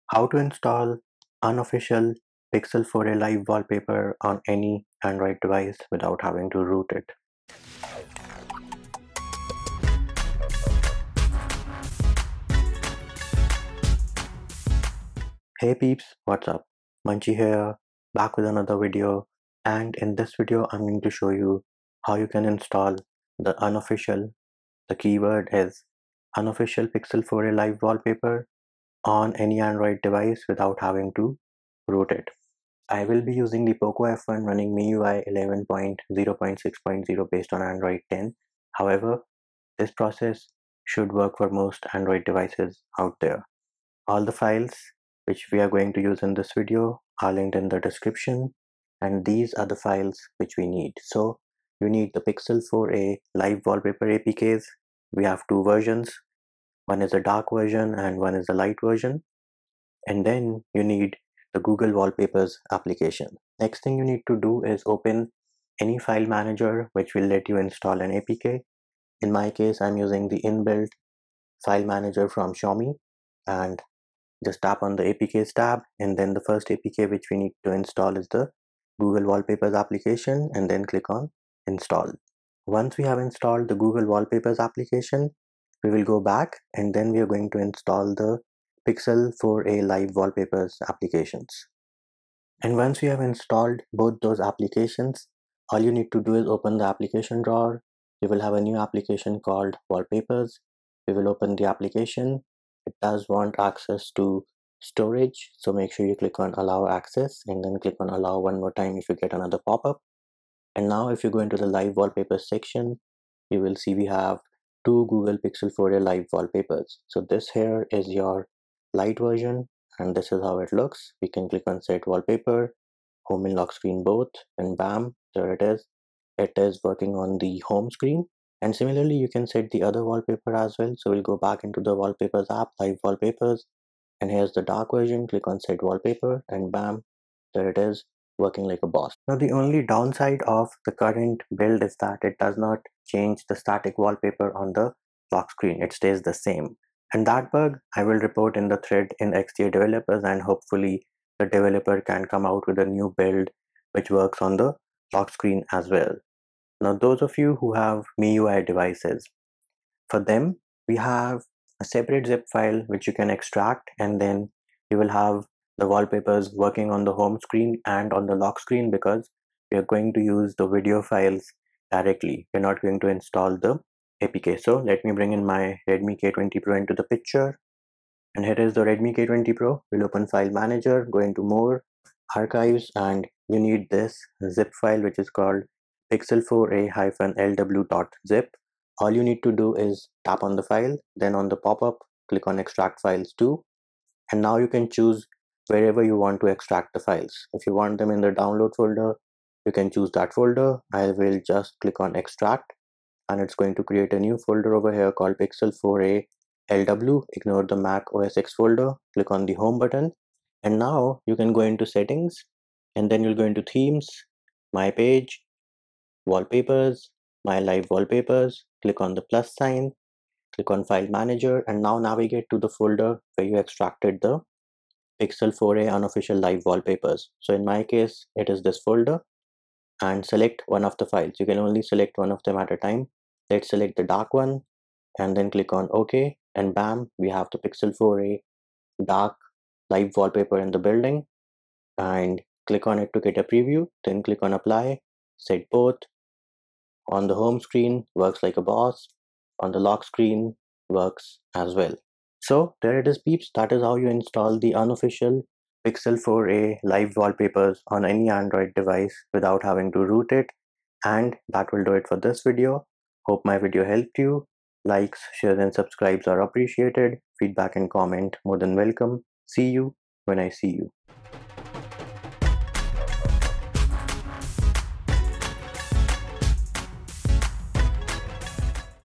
Install Unofficial Pixel 4a Live Wallpapers on Any Android (Without Root)
Do you want to learn how to install Unofficial Pixel 4a Live Wallpapers on any Android device without having to root it? In this detailed video tutorial we will show you how to install Unofficial Pixel 4a Live Wallpapers on any Android device without having to root it. We are going to use Poco F1 & Redmi K20 Pro running MIUI 11 based on Android 10 in this video.

**Buy Xiaomi Mi 9T Pro/Redmi K20 Pro**
**Flipkart**